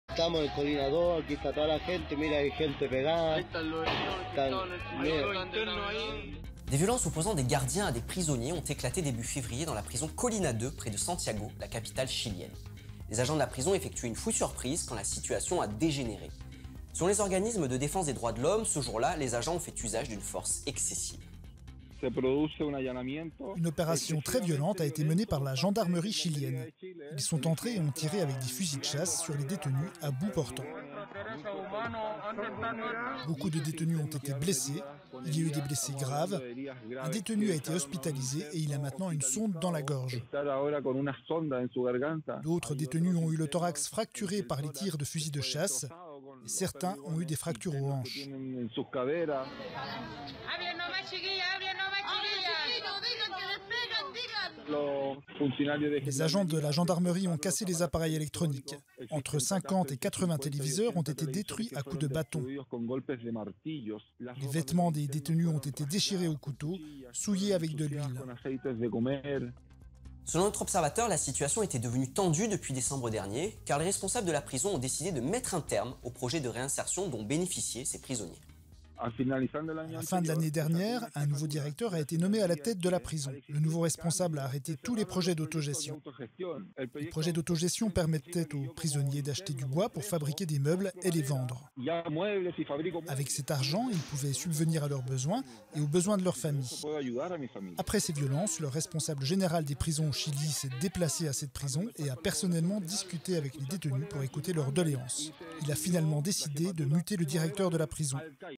Far-west en pleine prison au Chili après une fouille surprise
Des violences opposant des gardiens à des prisonniers ont éclaté début février dans la prison Colina II, près de Santiago, la capitale chilienne. Les agents de la prison effectuaient une fouille surprise quand la situation a dégénéré.